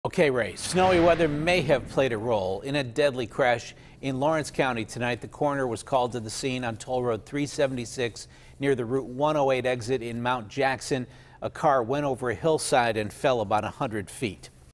1 Dead After Lawrence County Crash
One person is dead after a crash in Lawrence County.